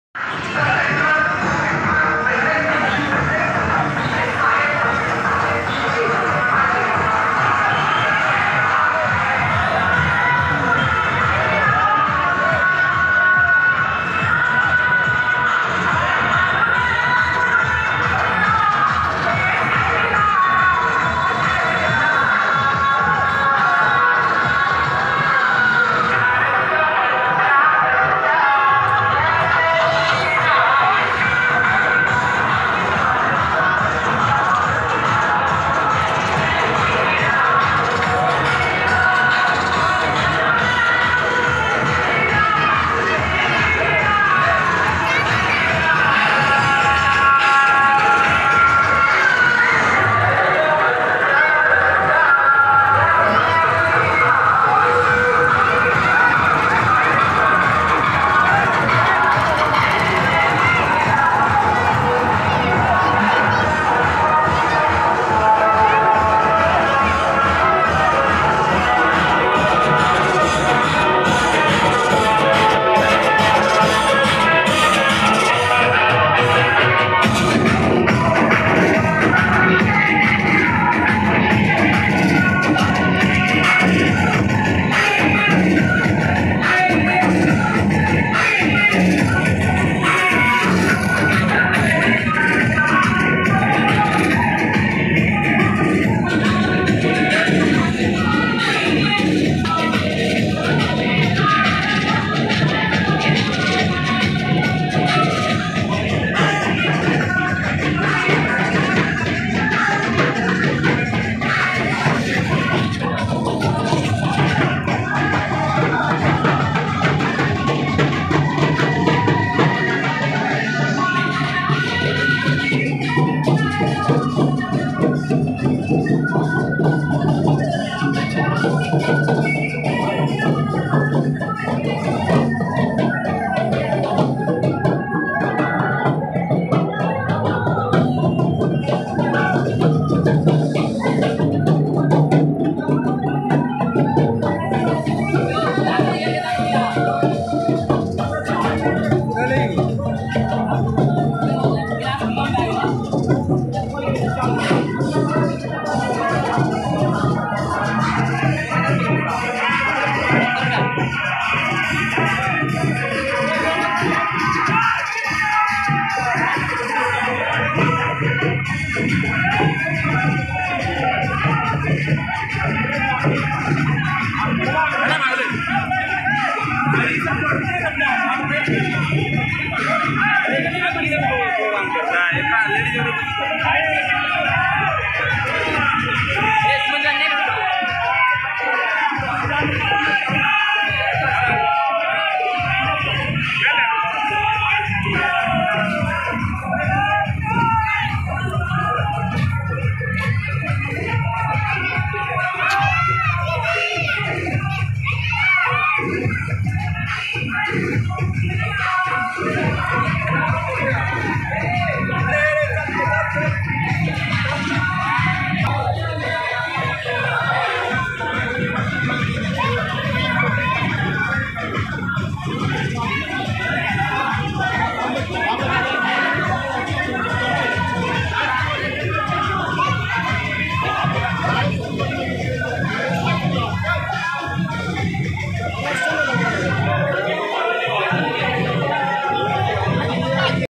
महथावा बजार (अररिया जिला) में महावीर जयंती के शुभ अवसर पर बजरंग दल लाखों की संख्या में 😱🚩👍
Hit likes.

Lots of ❤️🥰